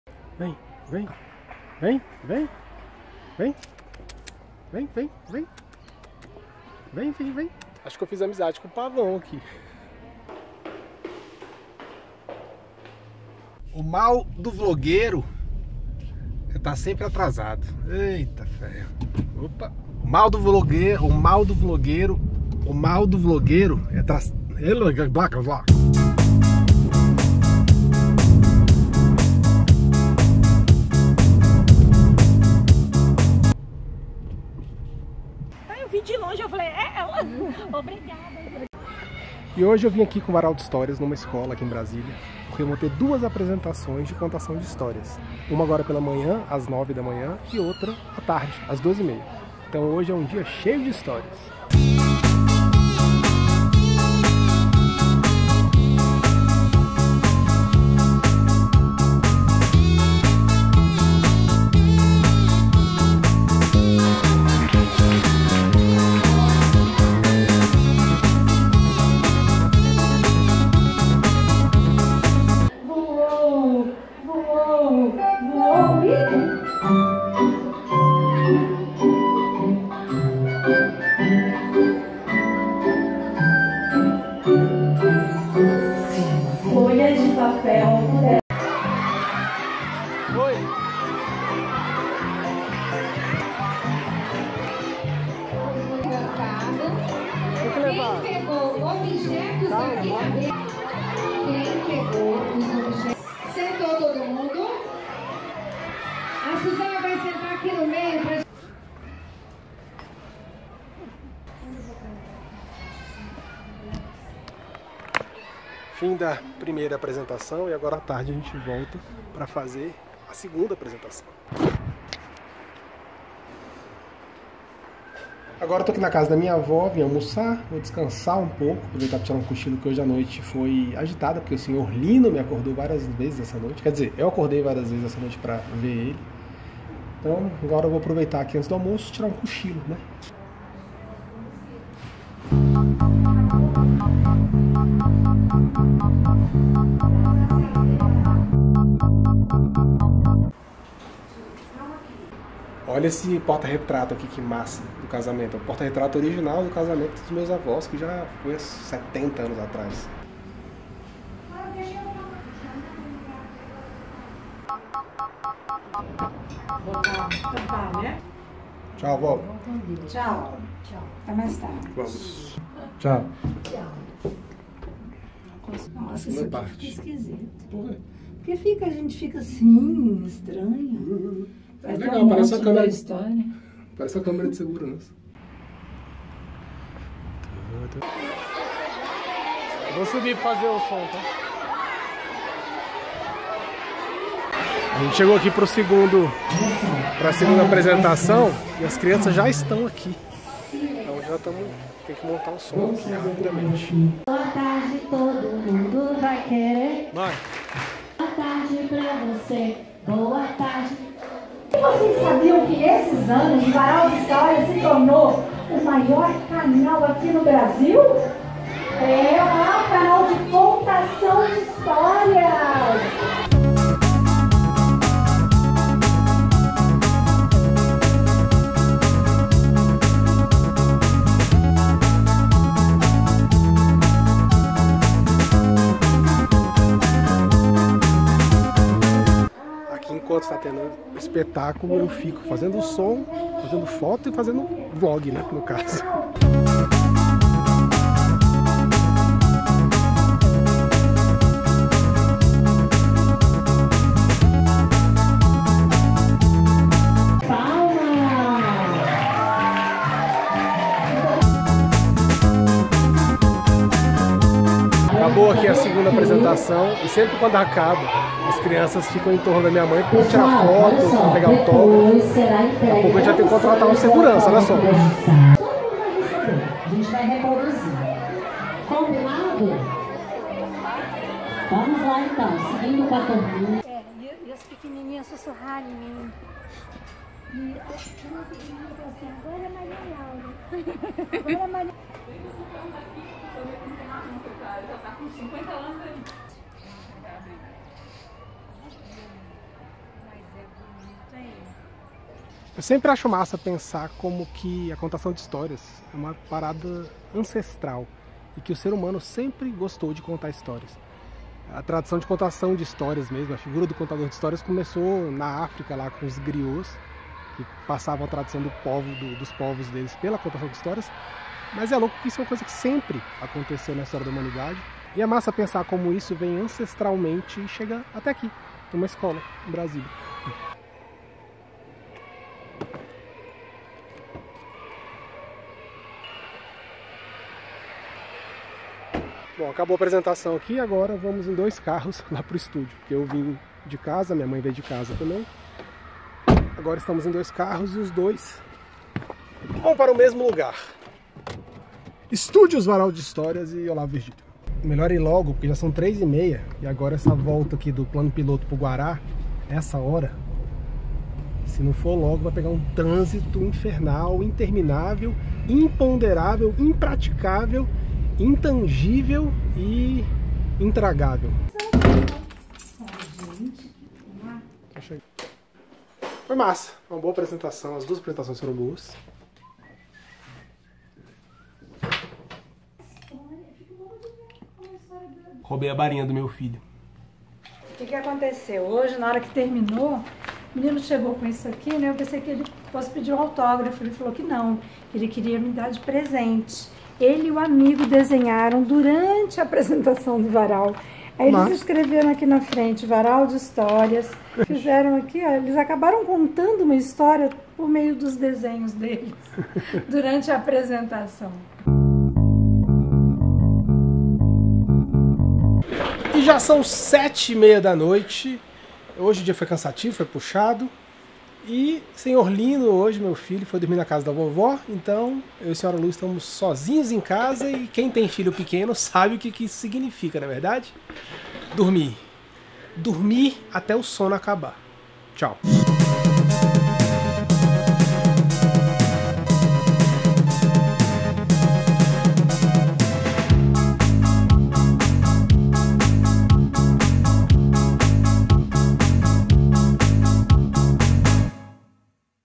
o MELHOR lugar para dormir - VLOG DE CRIATIVIDADE 41
Hoje foi um dia cansativo e muito massa! Fizemos duas apresentações com o @varaldehistorias em uma escola, aqui em Brasília.
A trilha sonora desse vlog é minha e vc pode ouvir ela
Equipamento usado:
- Canon 80D
- Tokina 11-16mm 2.8
- Rode VideoMicro
- Samsung S7